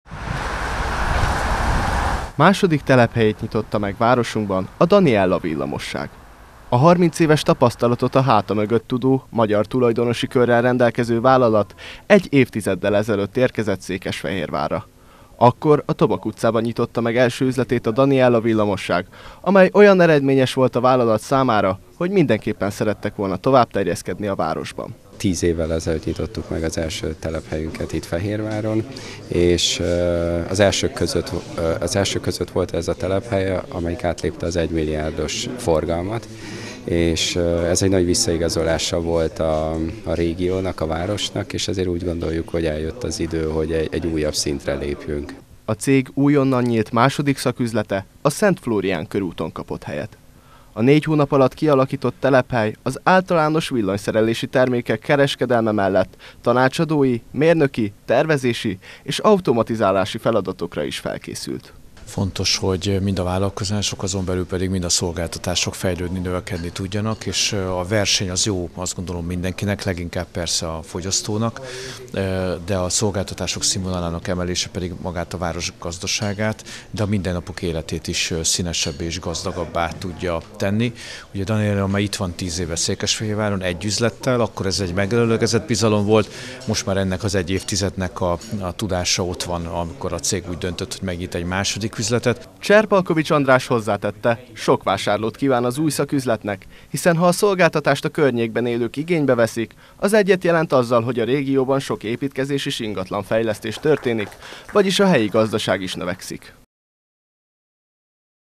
ÚJ TELEPHELY NYÍLT A SZENT FLÓRIÁN KÖRÚTON
Új telephelyet avatott fel szerdán a Daniella Villamosság a Szent Flórián körúton. Magyaroroszág egyik legmghatározóbb villamossági kis és nagykereskedelme új szaküzletével 5 munkahelyet teremt a városban.